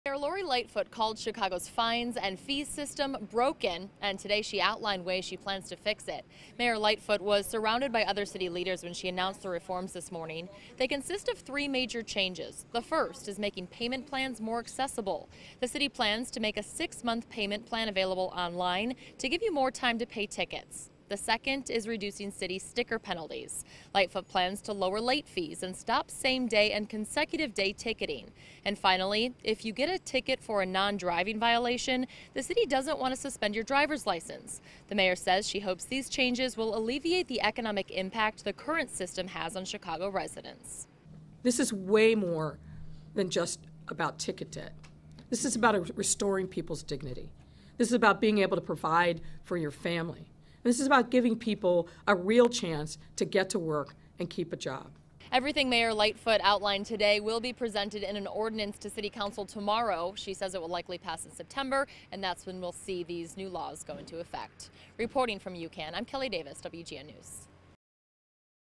Lightfoot proposes changes to Chicago's fine and fee system
Changes may be coming to the city's fine and fee system.